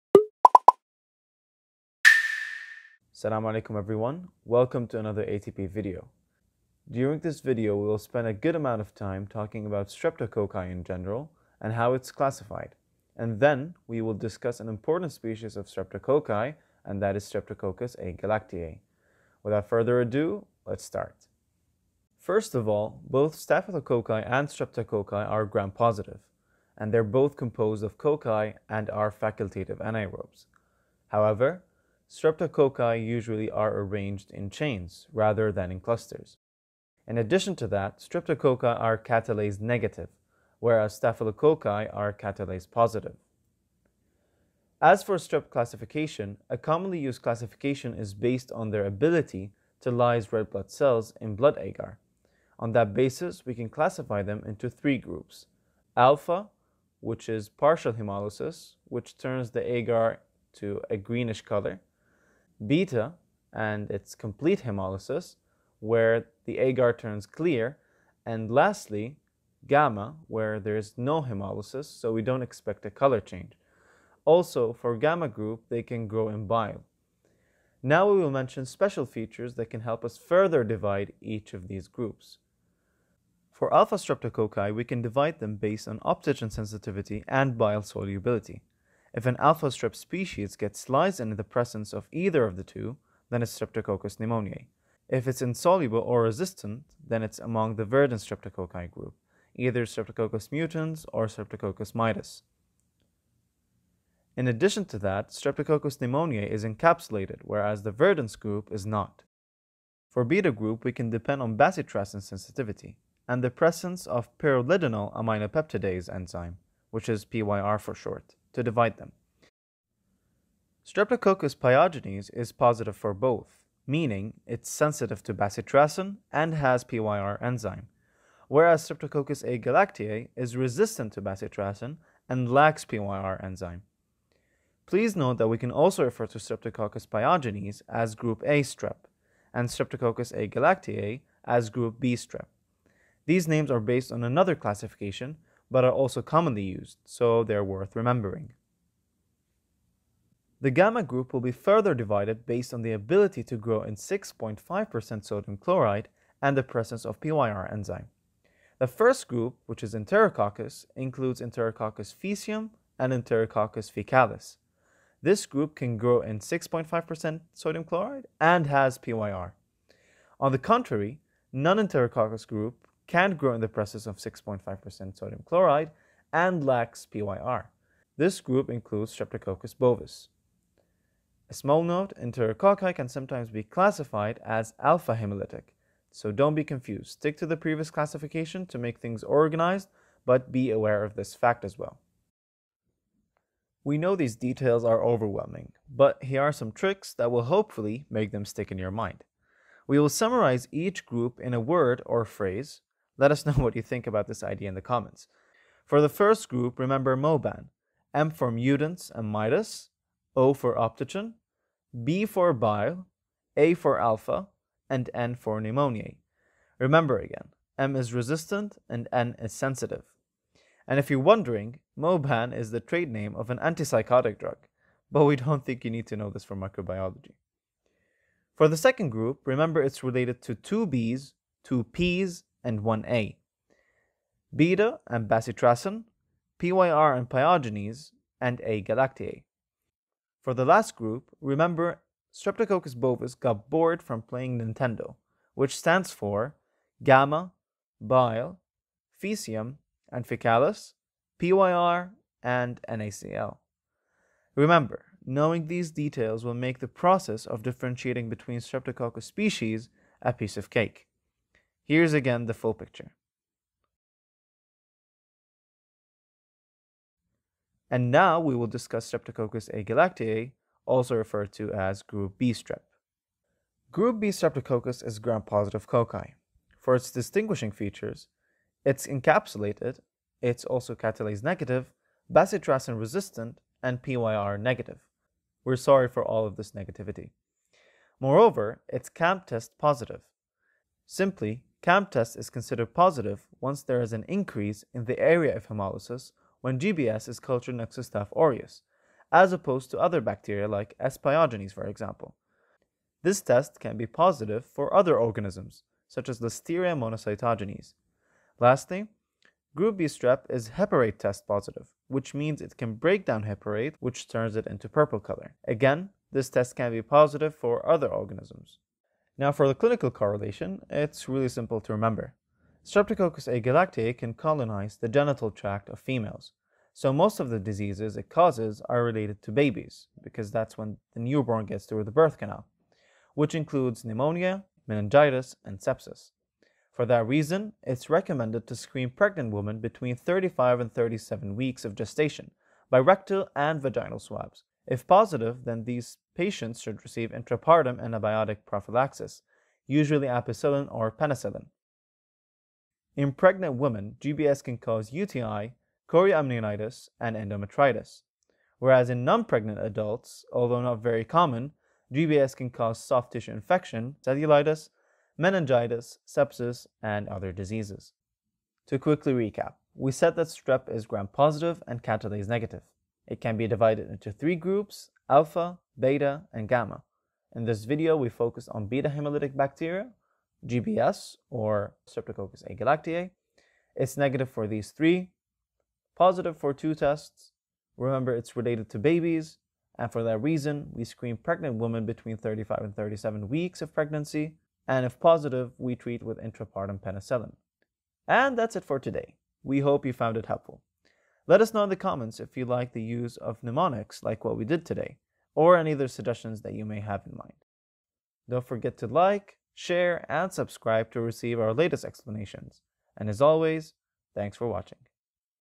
Gram Positive Bacteria: Streptococci and Group B Streptococcus (Streptococcus Agalactiae)
It is all positive today! This time around we will explore a new group of gram-positive bacteria. Then we will focus on a bug that is of great importance to pregnant ladies; GBS! Sounds cool, isn't it? Enjoy!

Contents:
0:23 - Streptococci vs Staphylococci
0:44 - Streptococci classification
1:23 - Alpha hemolytic streptococci
1:50 - Beta hemolytic streptococci
2:30 - Gamma hemolytic streptococci
3:14 - mnemonics
4:27 - Streptococcus Agalactiae (group B streptococcus)
6:08 - Summary

Credits:
- Arabic Subtitles: Radwan Alkhatib
- Illustrations: Anas Idris
- Script: Mohamad S. Alabdaljabar
- Video Editing: Anas Idris & Mohamad S. Alabdaljabar
- Voice Over: Abdullah Tajeddin